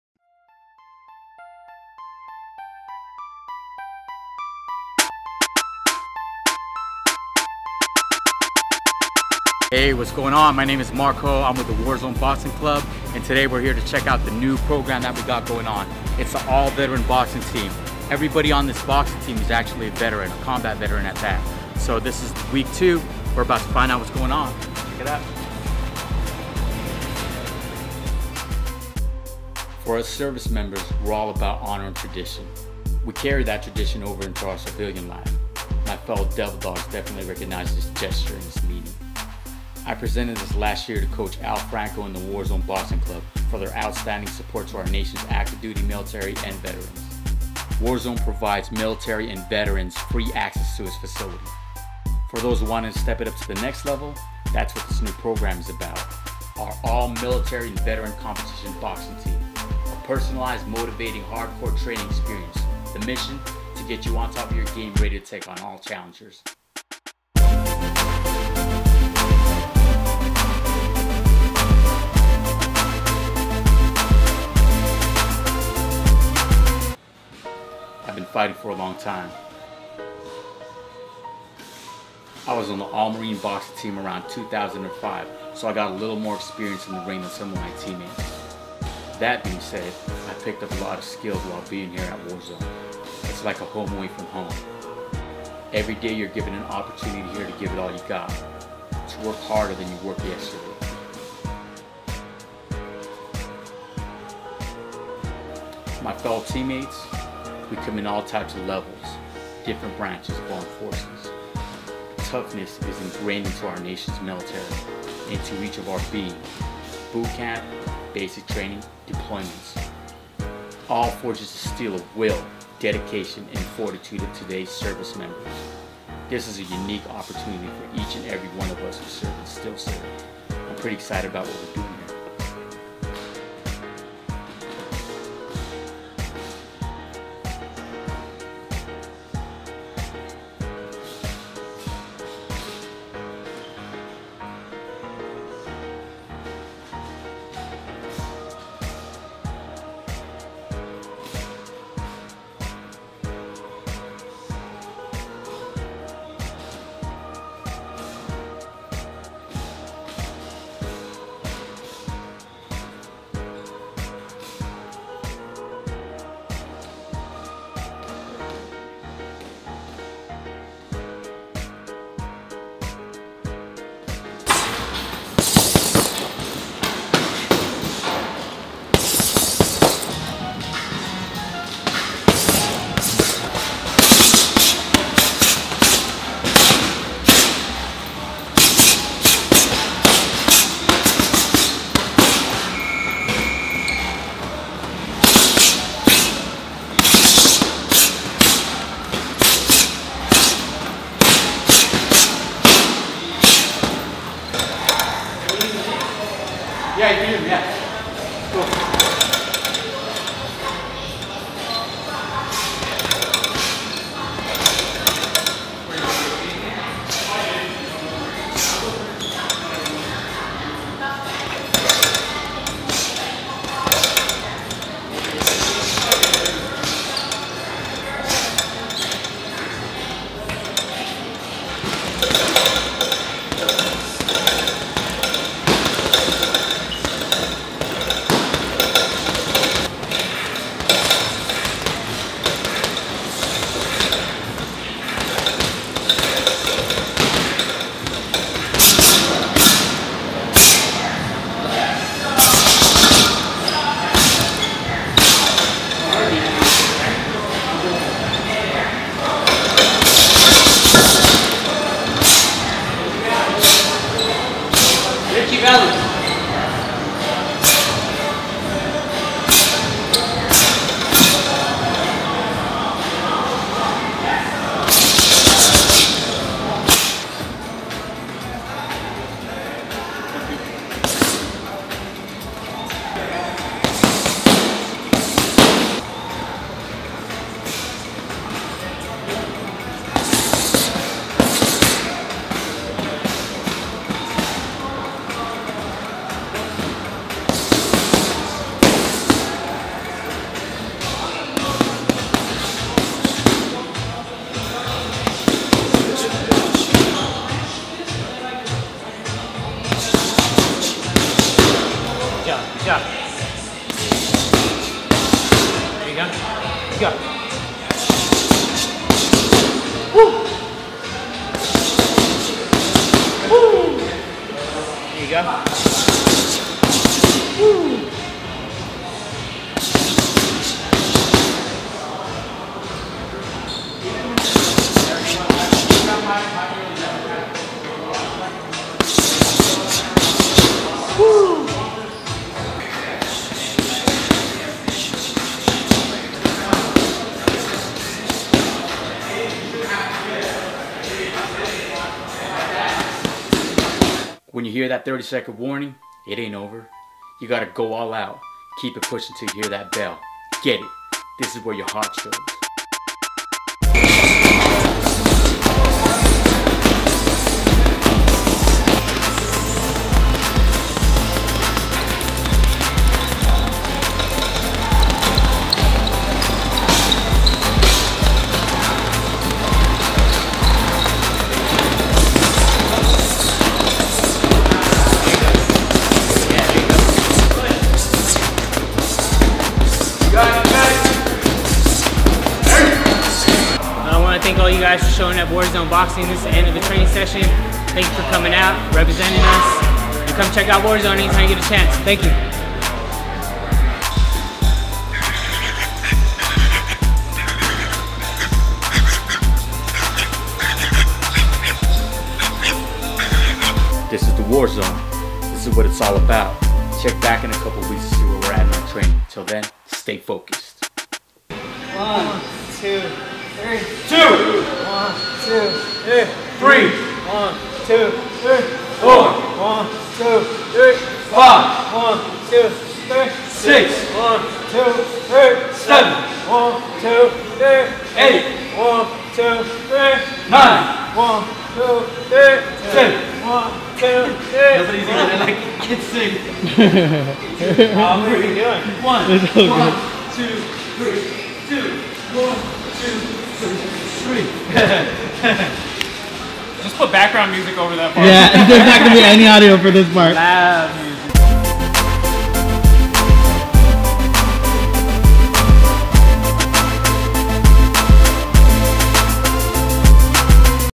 WarDogz Week 2
Video series about the newly formed Competition fight team "WarDogz" of Warzone Boxing Cub. An All Military/Veteran Fight Team ready to get it on.  If you've ever wondered about what it takes to be in a competition boxing team from start to finish, tune into this bi-weekly series!

Series created by BagYo and MervinLuwee
Music produced by: DieVerse
Follow him on Twitter: @DieVerseCA